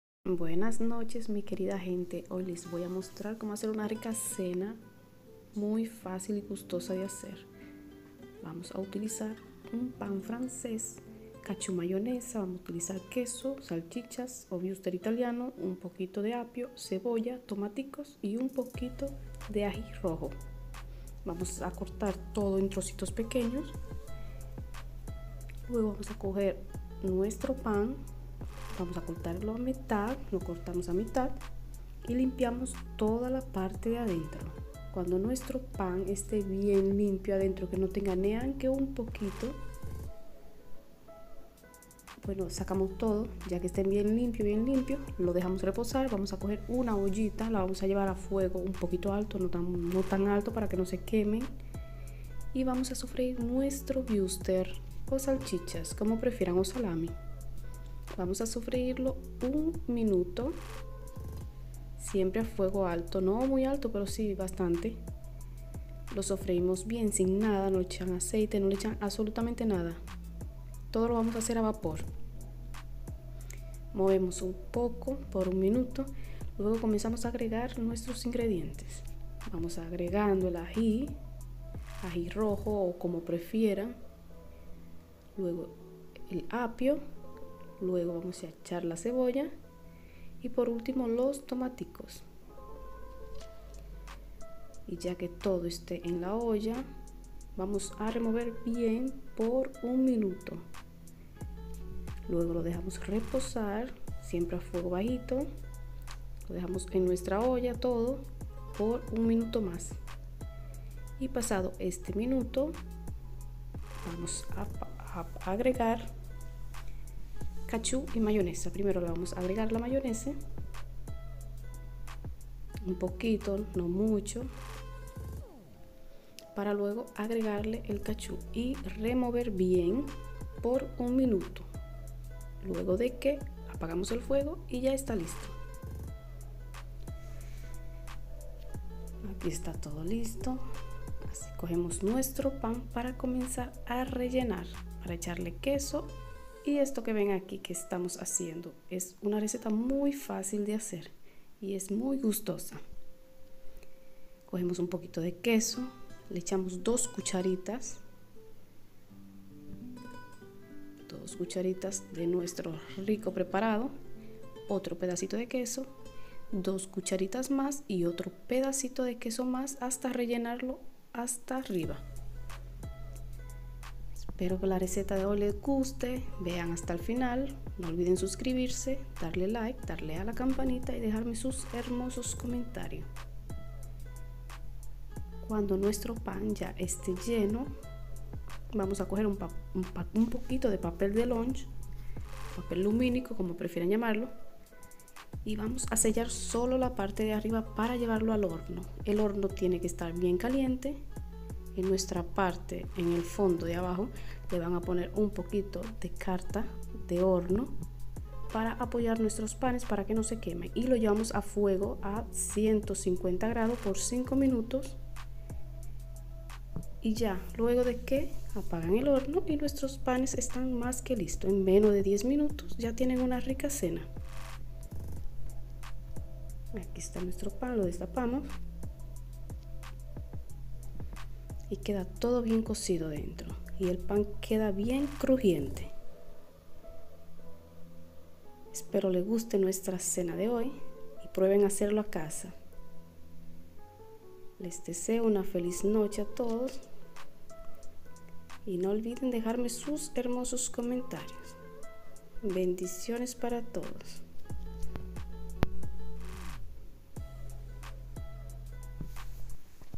Una Delicia en 5 Minutos
Receta de hoy fácil y sencilla 😋 , el día de hoy les vamos a mostrar cómo hacer unos ricos panes francés el famoso Baguette relleno con Wuster Italiano y otras cosas más si quieres saber más sobre este relleno quédate con nosotros hasta el final del vídeo.